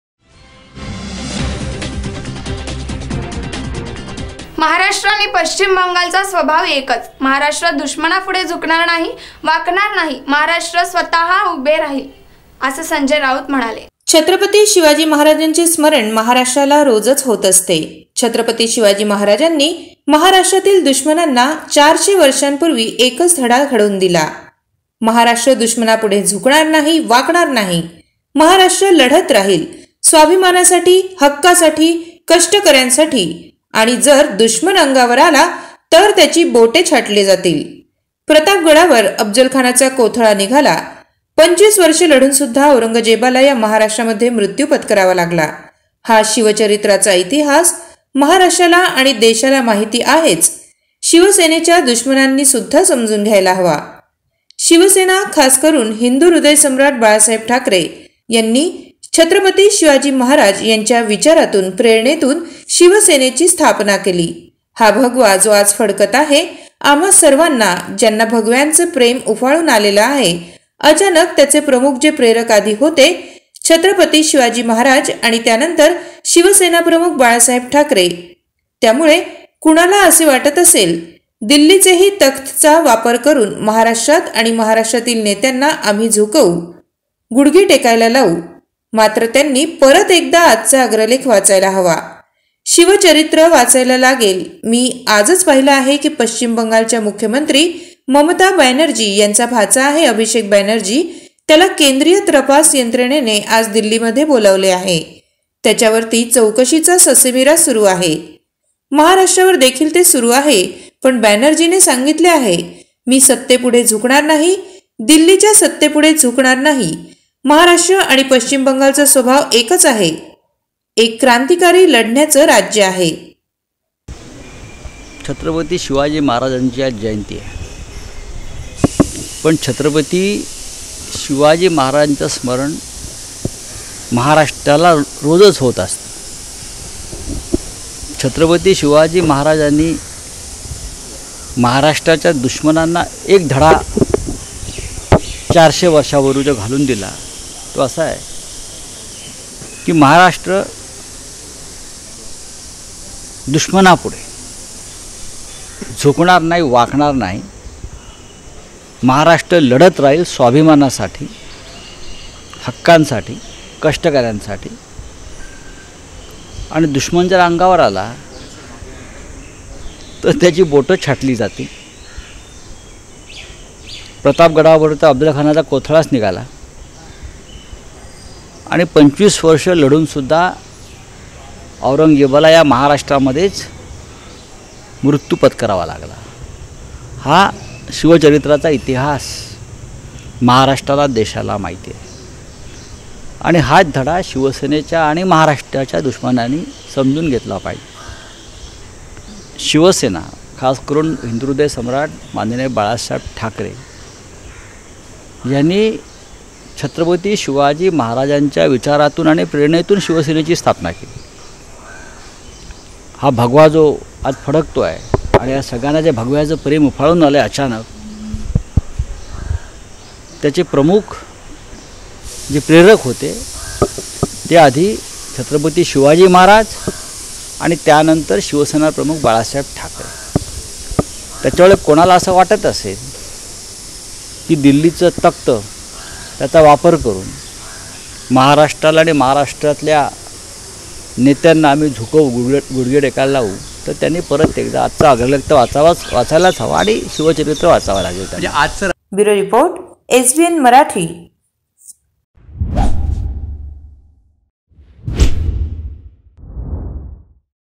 SANJAY RAUT | महाराष्ट्र आणि पश्चिम बंगाल चा स्वभाव एकच, महाराष्ट्र दुश्मना पुढे झुकणार नाही
महाराष्ट्र आणि पश्चिम बंगाल चा स्वभाव एकच, महाराष्ट्र दुश्मना पुढे झुकणार नाही, वाकणार नाही. महाराष्ट्र लढत राहील- संजय राऊत